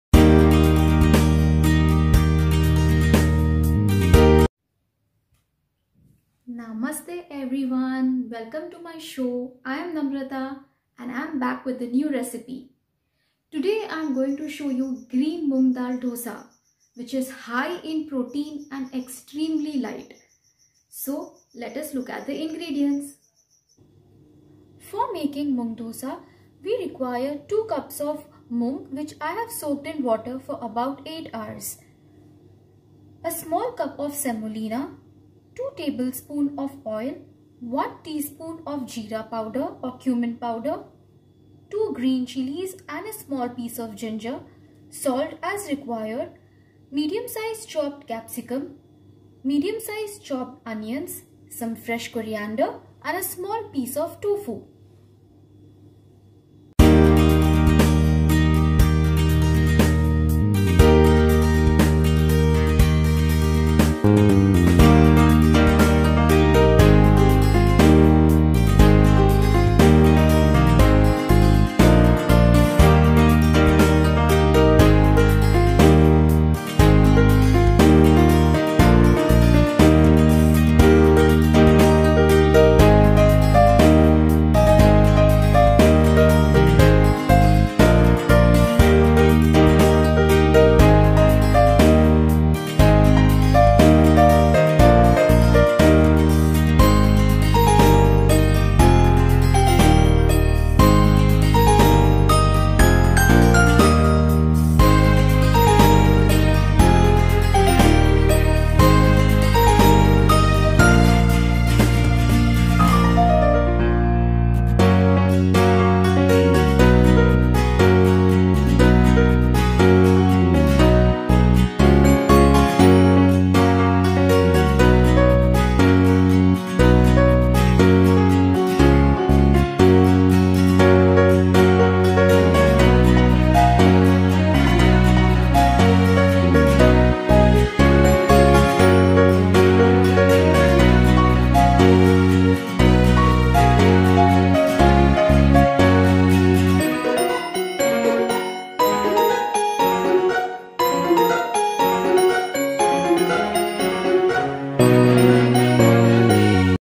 Healthy Breakfast recipe| Green Moong Dosa
Ingrediants

Boiled Green Moong/Beans - 2 cups
Green chillies & Ginger - 1 TSP
Salt as required
Capcicum - 1 medium size
onion - 1 medium size
Coriander
Tofu- 100 gms

Green moong is one of the healthiest source of plant protein. They are also rich in vitamins & minerals. They contain essential amino acids that your body cannot create on it's own. They are extremely light. They are high in fiber and helps to keep your bowels regular by speeding up the movement of food through your gut.

Moong can be eaten in many ways. It ca  be eaten in form of dosa,stir fry moong beans or as sprouts.